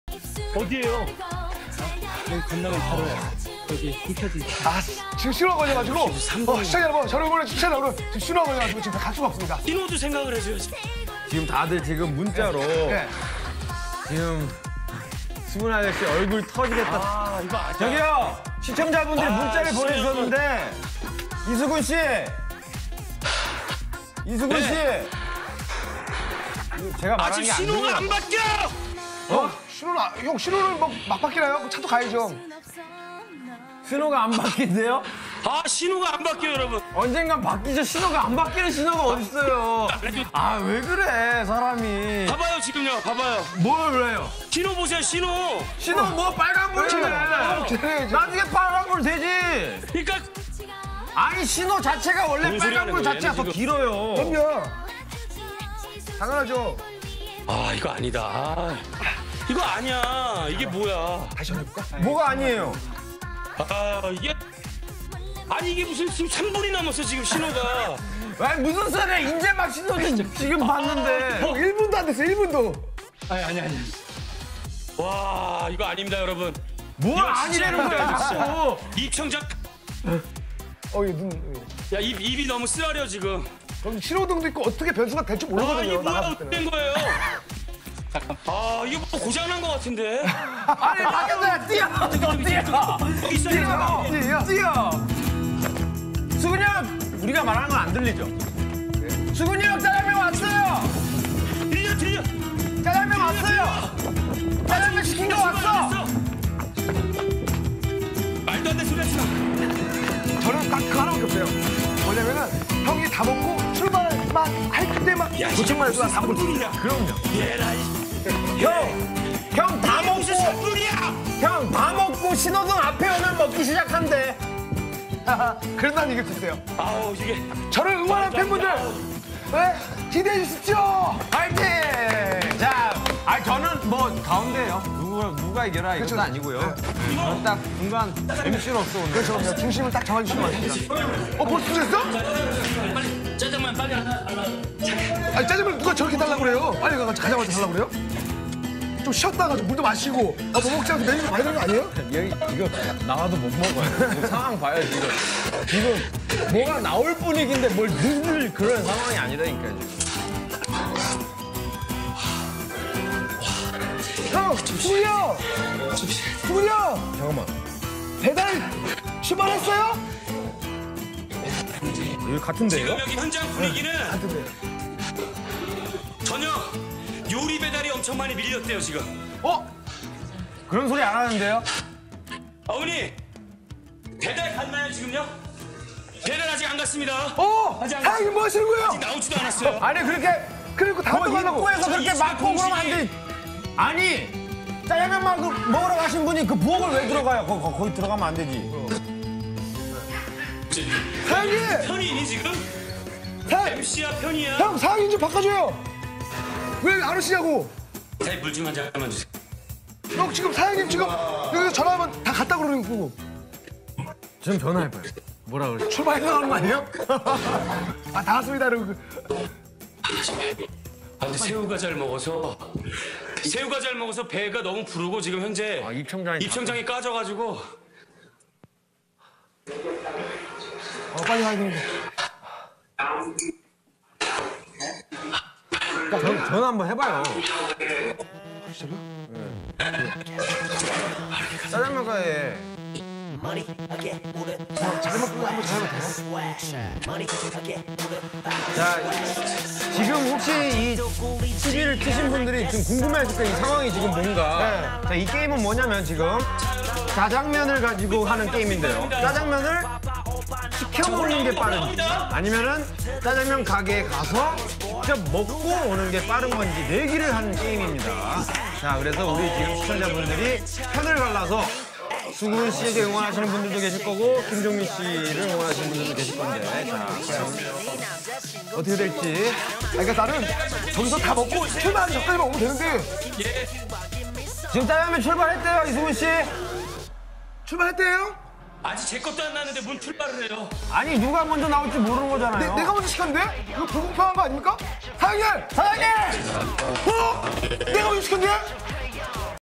배달 짜장면 VS 직접 가서 먹는 짜장면!_ 세 얼간이 3화
[세 얼간이  3화]

짜장면을 빨리 먹고 싶다면? 배달 짜장면 VS 직접 가서 먹는 짜장면! 본인이 선택한 사람이 이겼을 경우 영화 예매권이 선물로? 과연 승자는?